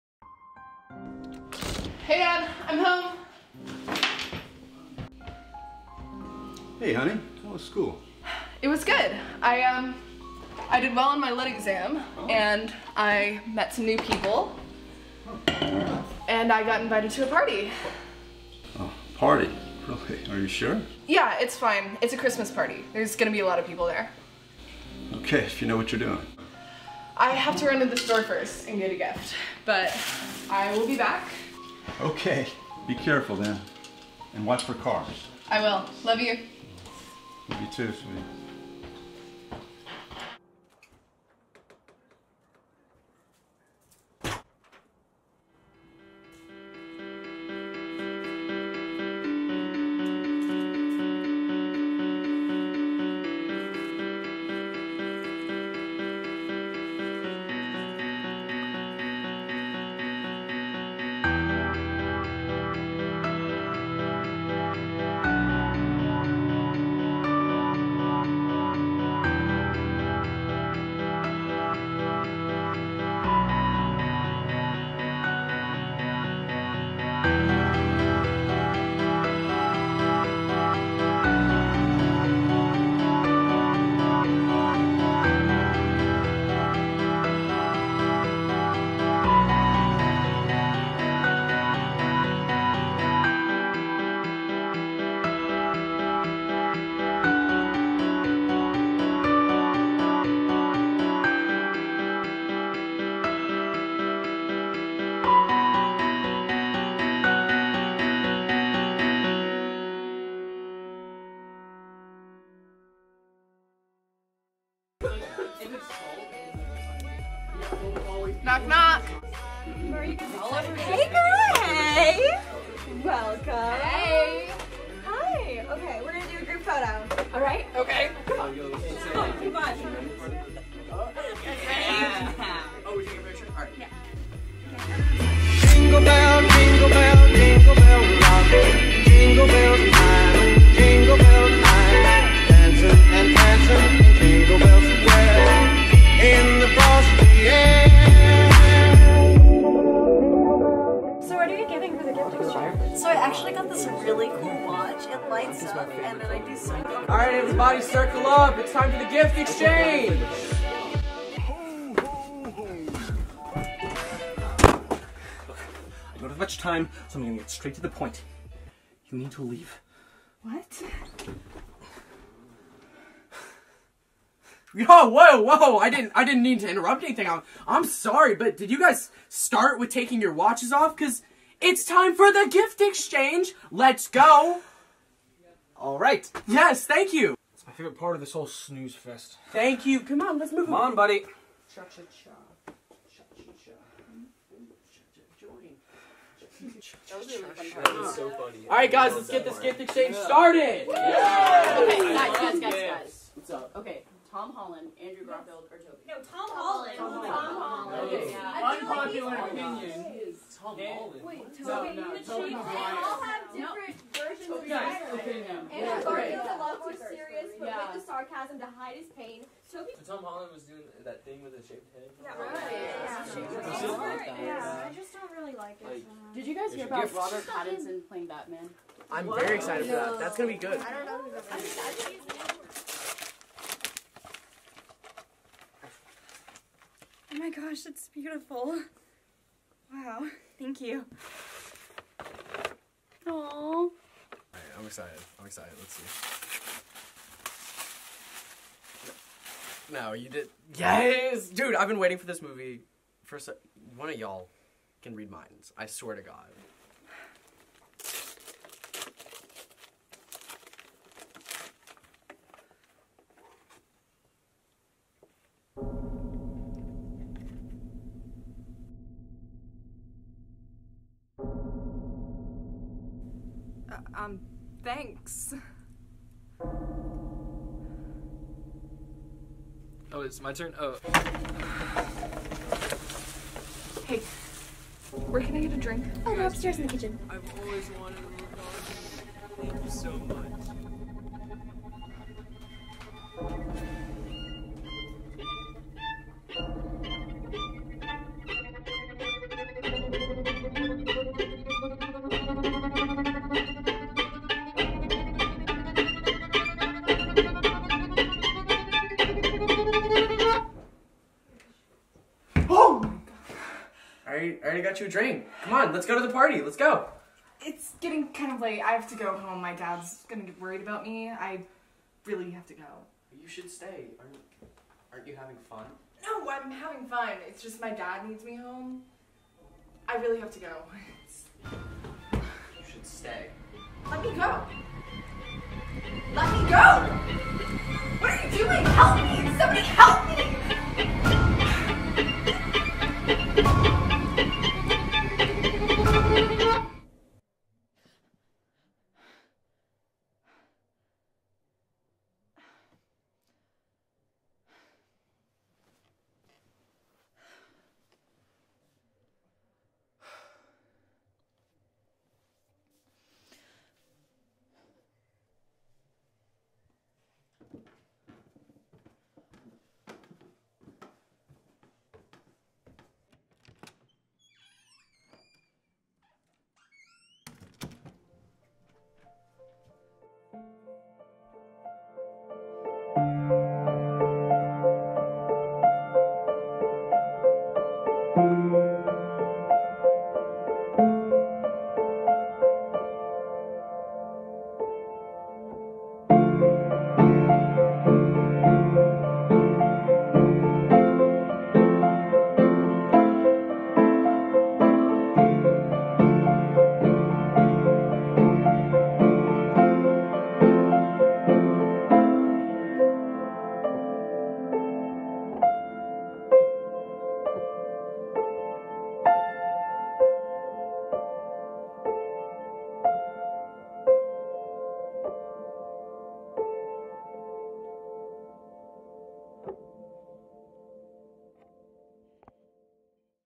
Welcome. | Thriller Short Film
Welcome. is about a new girl in town who is invited to a Christmas party, but what seems to be a normal party is really something darker. This film is nspired by the movie Get Out.

Thank you to my beautiful group who help put this together. We had the wonderful direction from Will Player, written and filmed by him and myself. It took two full months to make and produce, so thank you to all who put in their hard work at time to make this happen!

Crew:
Director/Editor- Will Player
Prod. Assistant/Costumes/Music- Vienne Borg
Audio/Props- Max Oglesby
Camera- Milan Robertson
Audio/Lighting- Naomi Borg and Cody Gage-Cole

Cast:
New Girl- Julia O'Neal
Friend- Joe Scott
Group Leader- Jackson Courtney
Party Goers- Vienne Borg, Milan Robertson, Jeffery Player, Liz Ortega, Cody Gage-Cole, and Lana Aga